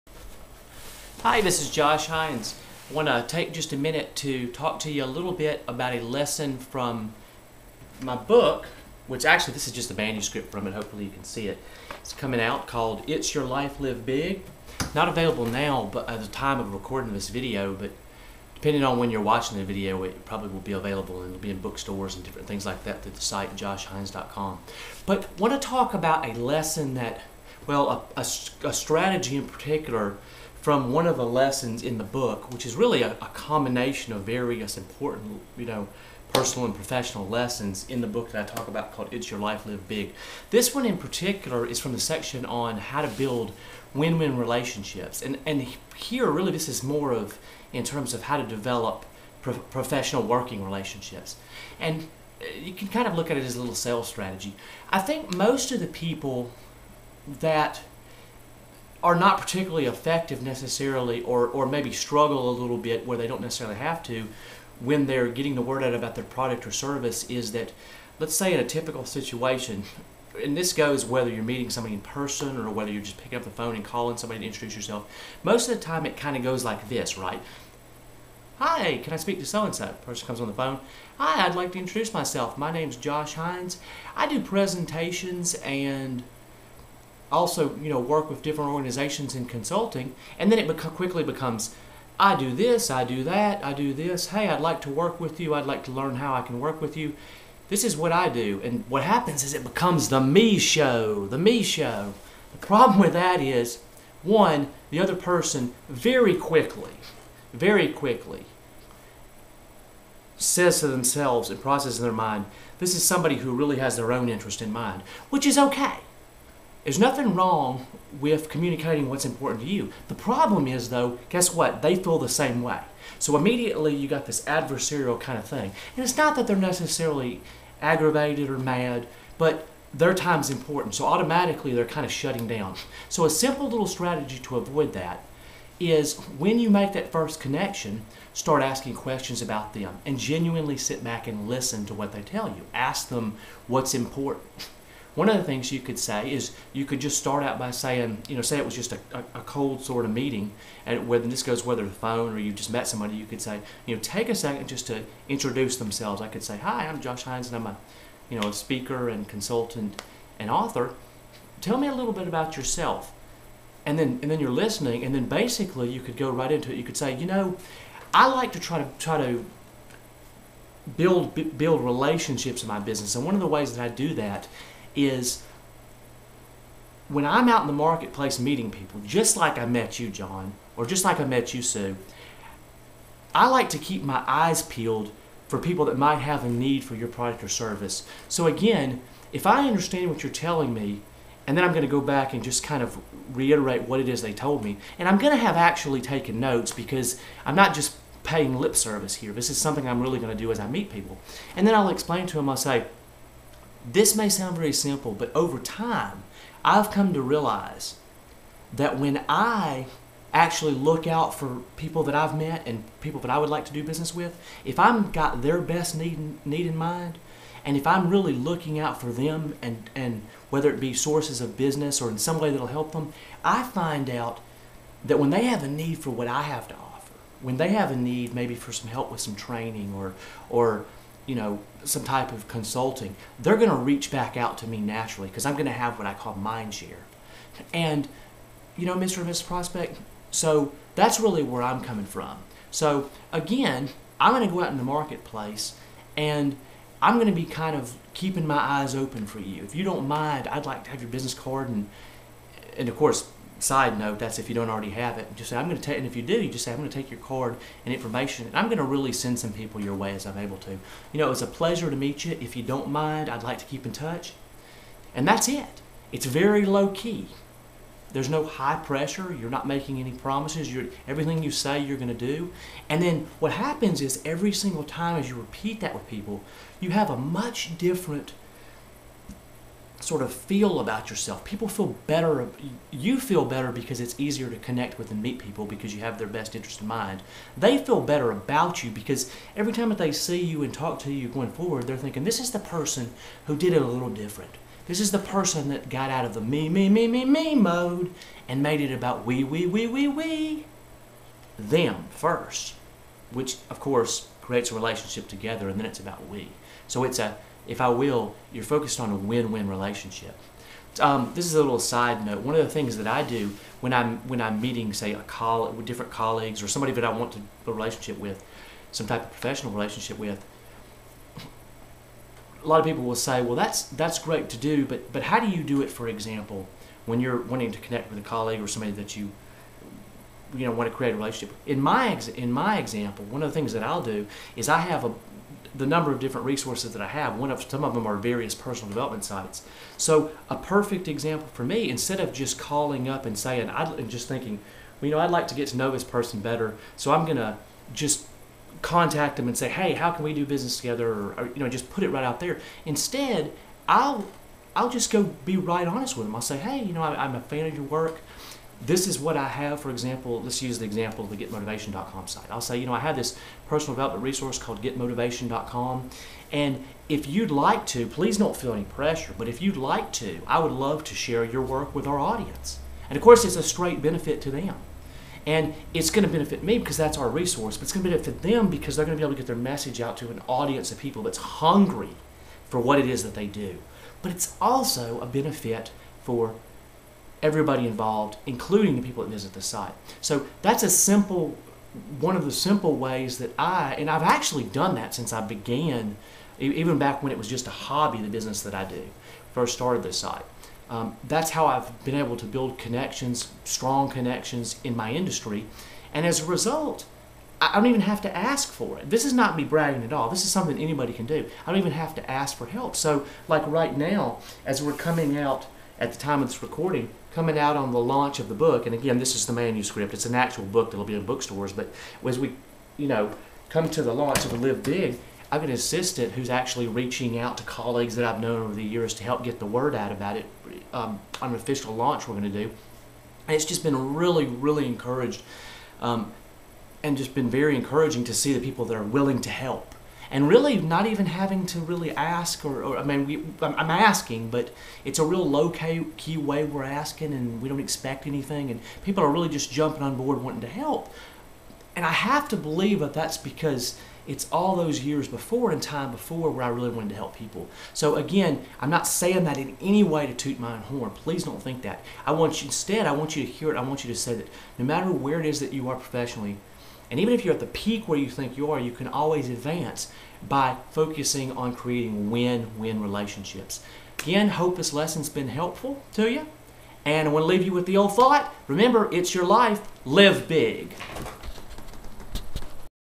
How To Create Win, Win Business Relationships By Josh Hinds
Learn how to develop win, win mutually beneficial business relationships with prospective clients and customers from author, consultant, and entrepreneur Josh Hinds. The ideas shared here are also helpful if you aspire to stand out as a leader in your industry, and or profession.